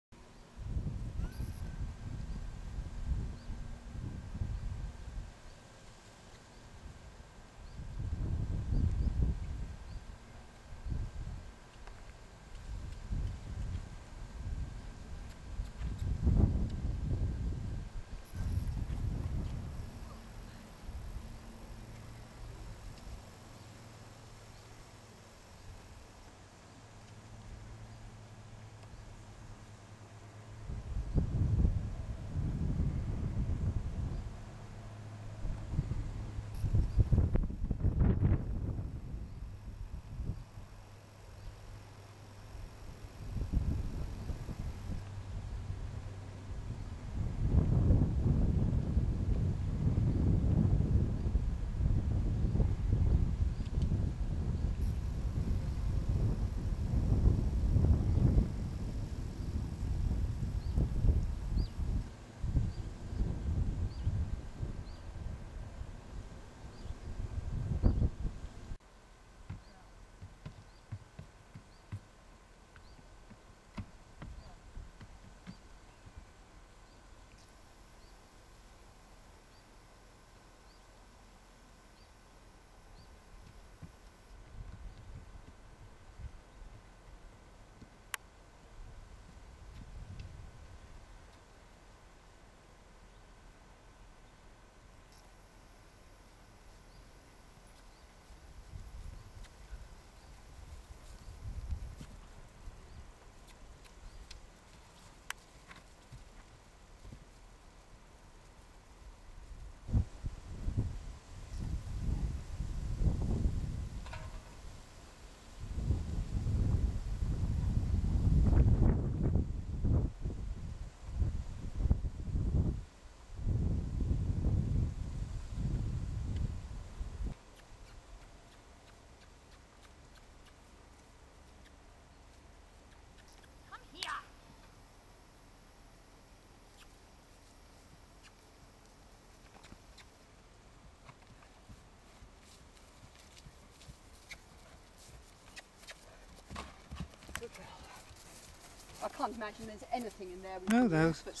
Horse Agility - Vanessa Bee and Secret
Secret and Vanessa Bee Playing at Horse Agility